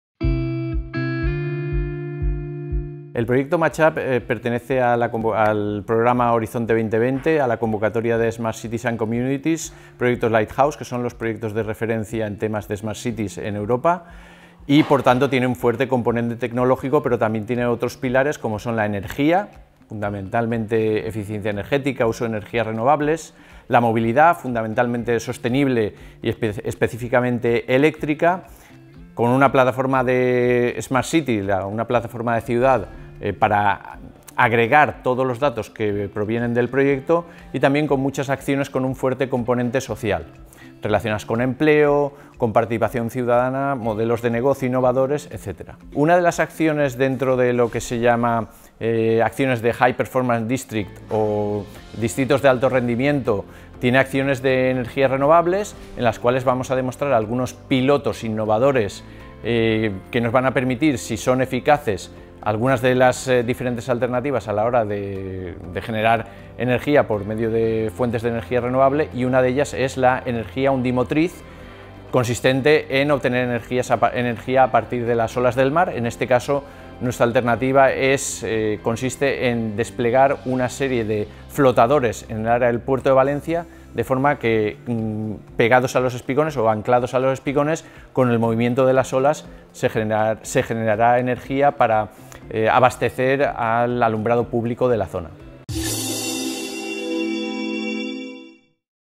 Ernesto Faubel, Coordinador de Proyecto MatchUp
Entrevista realizada el 2 de julio durante la Jornada sobre Emprendimiento Azul  y el Impacto de las Energias Marinas en el Mediterráneo organizado por CEEI Valencia, dentro del Proyecto MAESTRALE.
MAS INFORMACIÓN SOBRE EL EVENTO
MÁS INFORMACIÓN SOBRE MAESTRALE